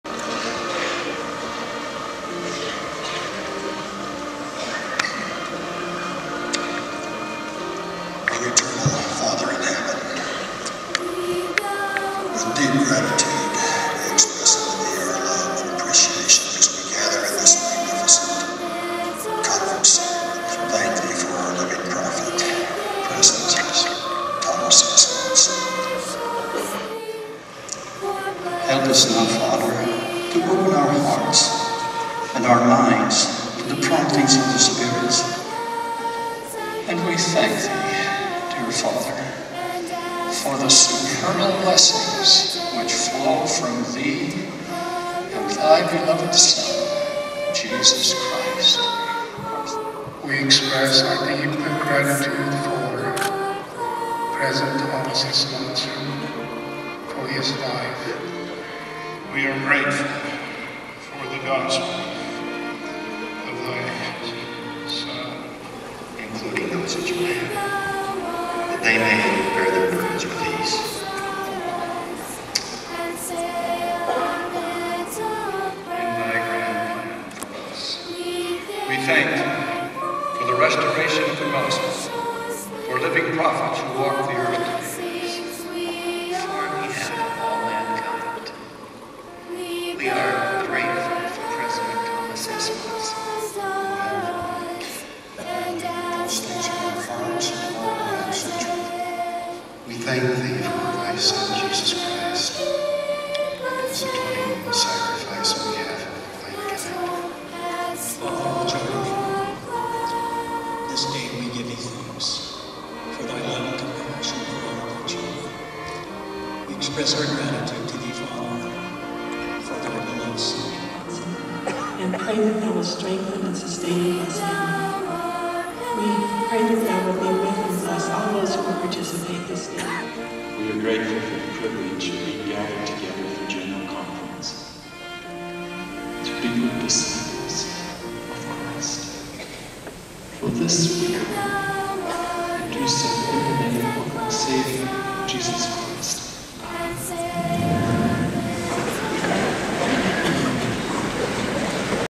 LDS Leader, President Eyring, during General Conference Opening Prayers
Watch LDS Leader, President Eyring, during the opening prayers of General Conferences from April 2009 to October 2014.  While other members of the First Presidency and Quorom of the Twelve had their eyes closed during the prayers, President Eyring always has his eyes open, often rifling through his notes or fidgeting around.  Pay attention to Eyring's reaction to the words "Thomas S Monson" in the first clip of the video -- you'll be surprised.

Normally, this would not be a big deal.  However, as a leader in one of the world's larger religions, President Eyring is held up as an example, as the LDS faith teaches its members from a young age to bow their heads and close their eyes during prayers.

President Eyring is currently at the Vatican, awaiting to speak.